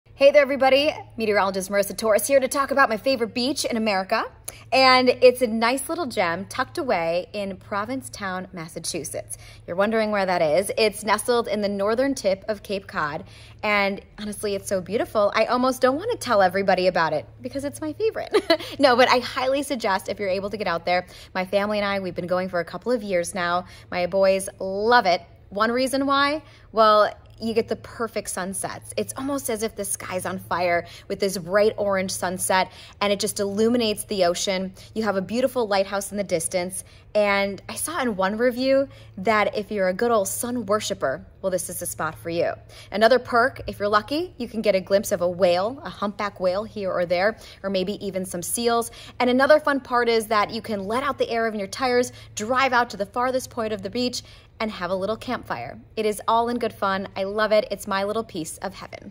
Vote For America's Top Beaches | Provincetown | FOX Weather Marissa Torres
FOX Weather Meteorologist Marissa Torres says Provincetown beach in Massachusetts is the most scenic in the U.S. Vote for Provincetown